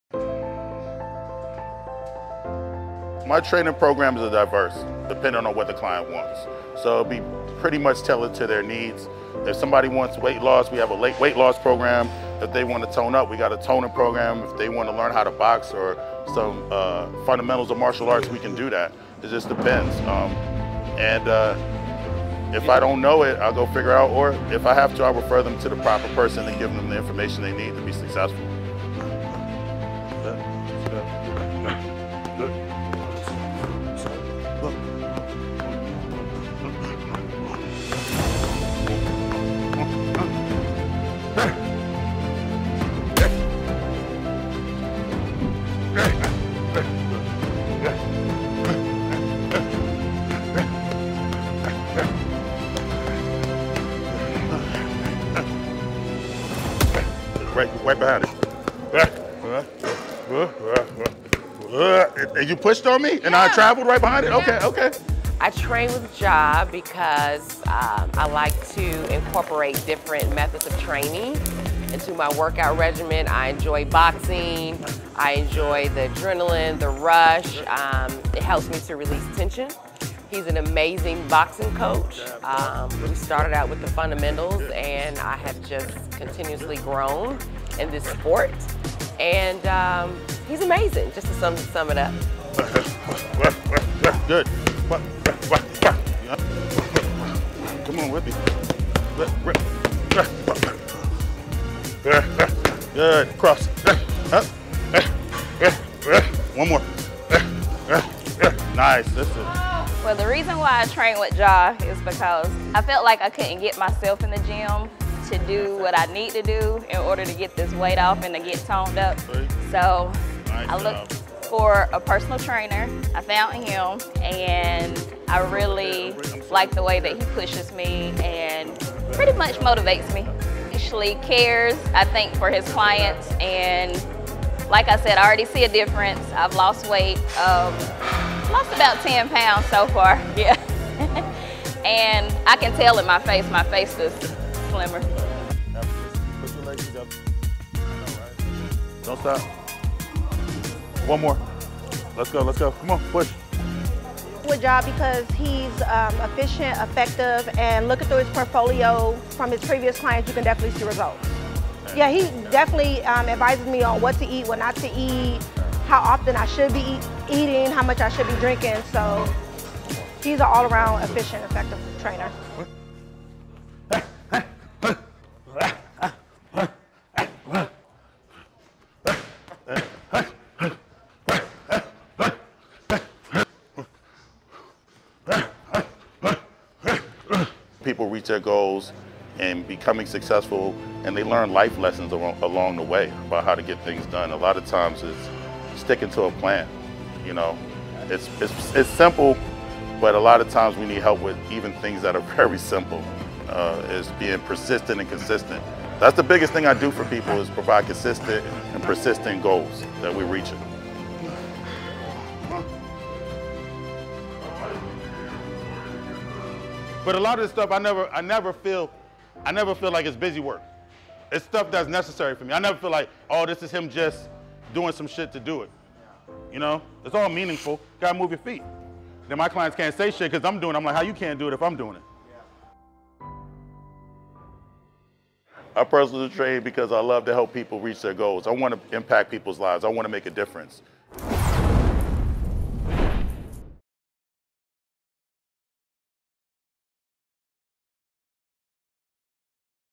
Training with Jah
Train with Jah Johnson. One of our personal trainers at The Office Gym.